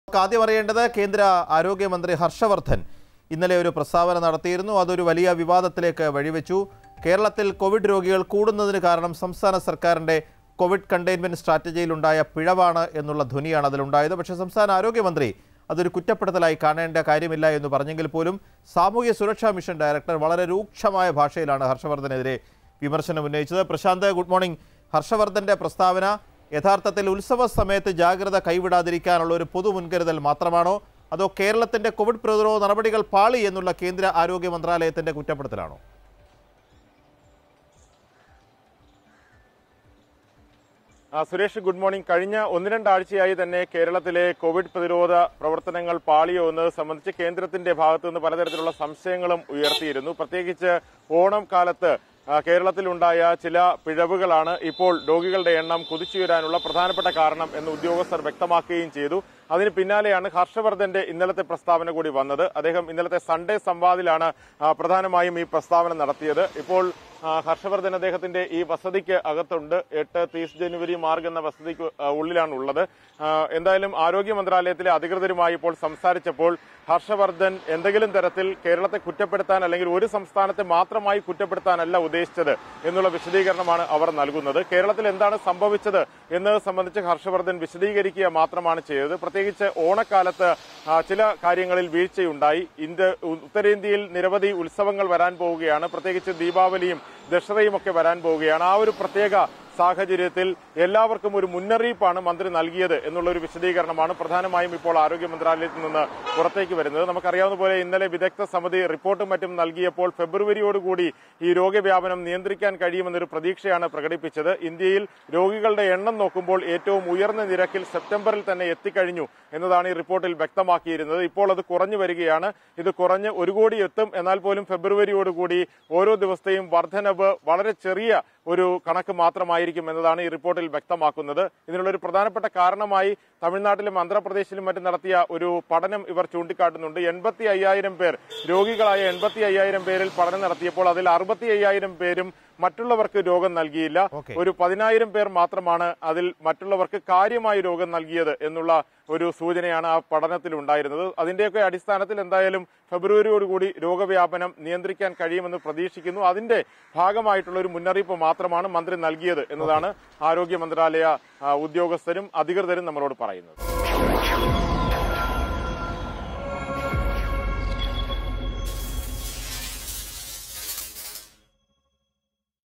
വിമര്‍ശനത്തില്‍ വിശദീകരണം ; കേരളത്തെ മാത്രമായി കുറ്റപ്പെടുത്തിയിട്ടില്ലെന്ന് കേന്ദ്രം | Asianet News
Harsh Vardhan has criticized Kerala coronavirus prevention activities controversy